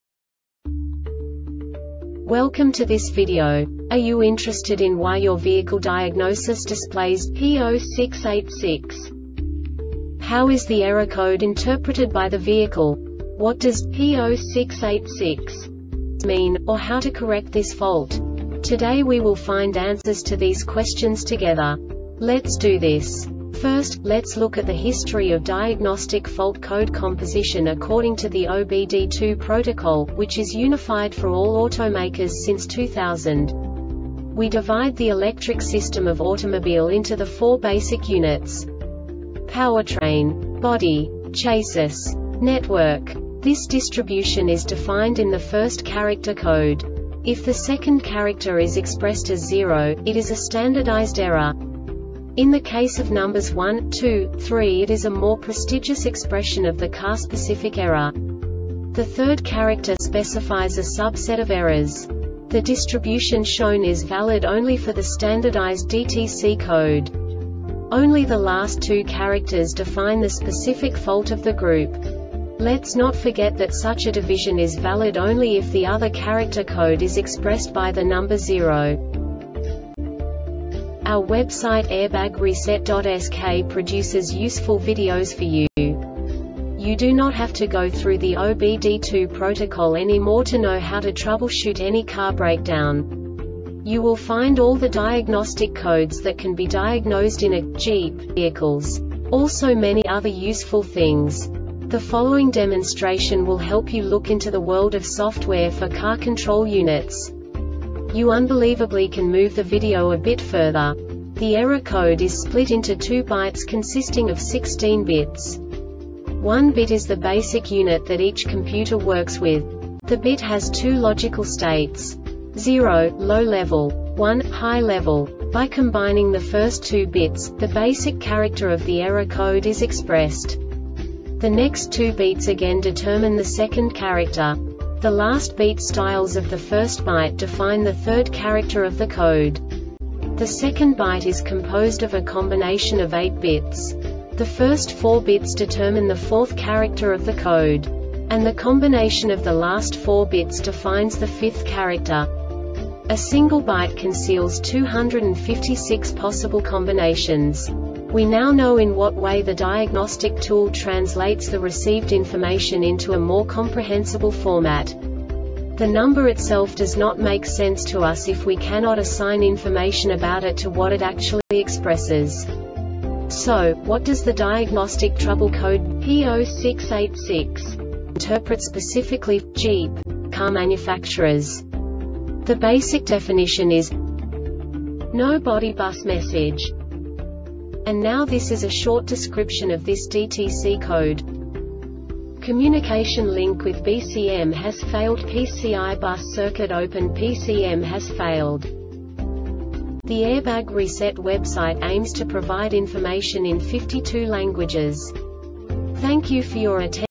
DTC Jeep P0686 Short Explanation
Contents:
0:21 Basic DTC analysis according to OBD2 protocol standard.
1:48 Insight into programming
2:58 A diagnostically understandable description of the Diagnostic Trouble Code
3:05 Basic error definition

Make:
Jeep
Code:
P0686
Definition:
No Body Bus Message
Cause: